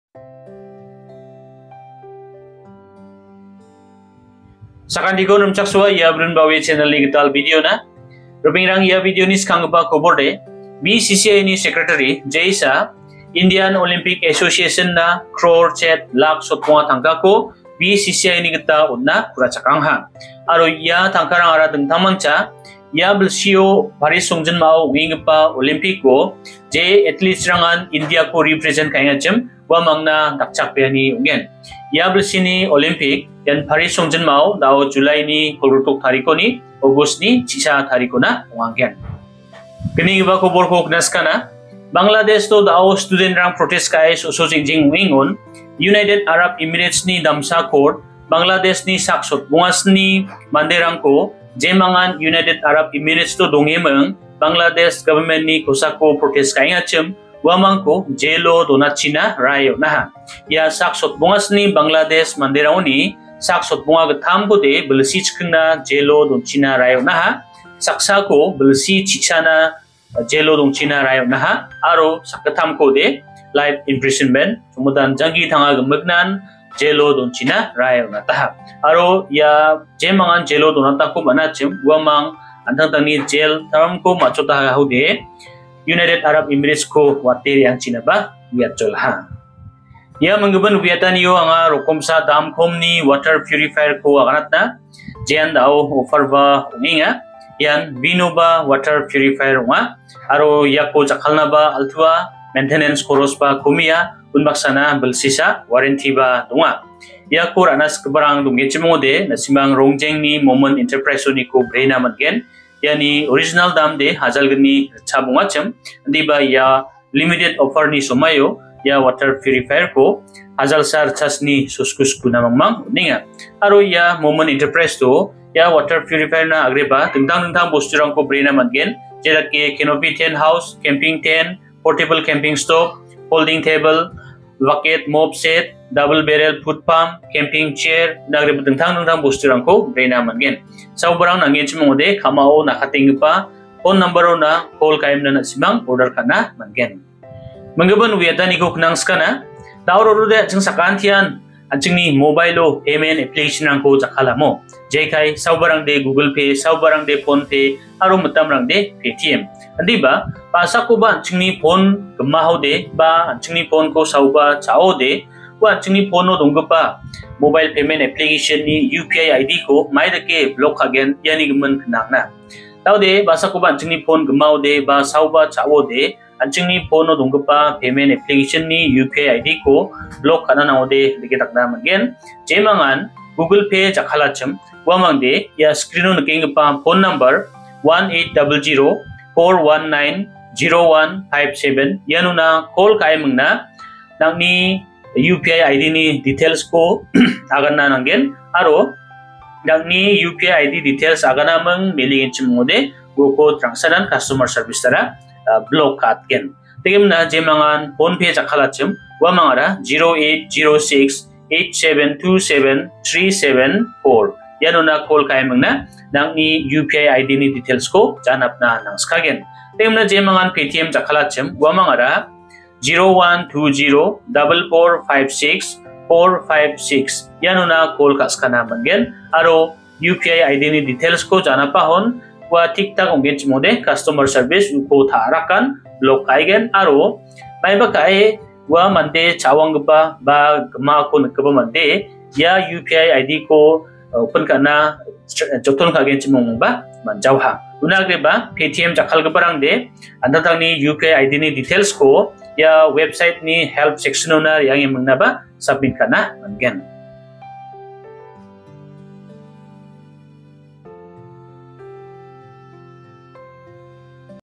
BCCI Give Rs. 8.5 Crore | Bangladesh Manderangko UAE Jailo Donata | UPI ID Block Ka·ani | BrinBawe
BrinBawe News
23 July 2024

We Do These:

1️⃣Fishing Competition Announcement

2️⃣Product Advertisement

3️⃣Business Enterprise Promotion etc...

Nikush Sangma